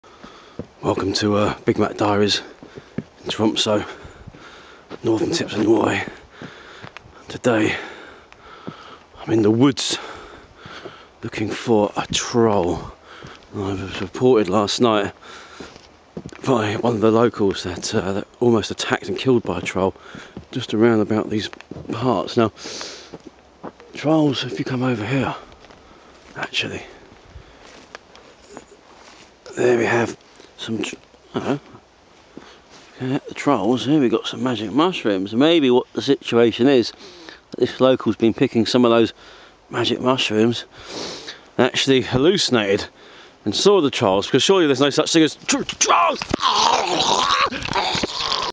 troll hunter
Big Matt Diaries was in Tromso, Norway, searching for Trolls. A local was attacked last night, and Big Matt Diaries went to investigate!